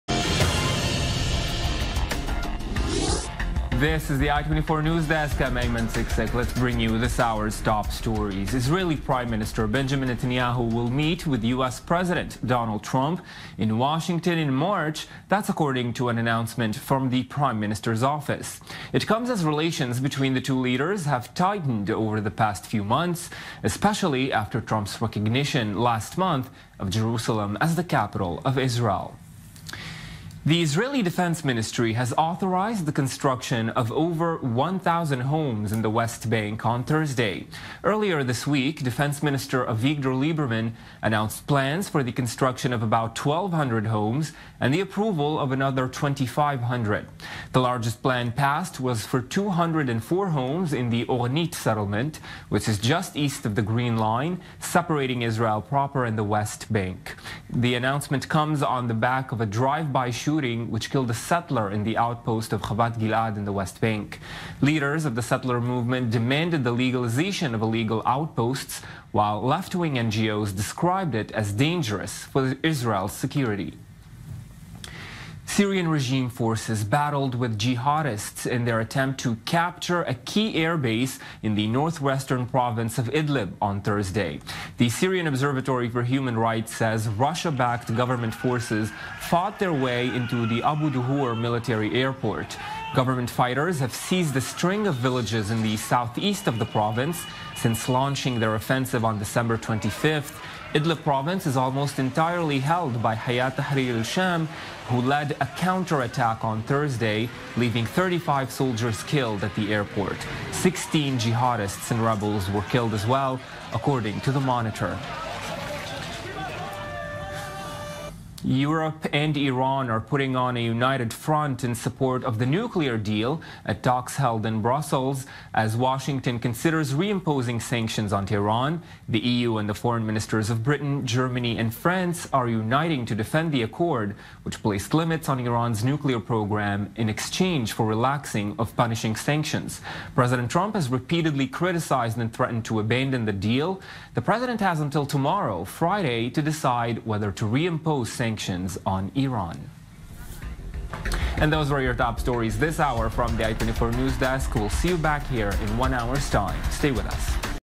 Top stories today
Top stories today with @aymansikseck: Netanyahu expected to meet Trump in March, 1000 new homes approved in the West Bank, Syrian regime fights jihadists for an airbase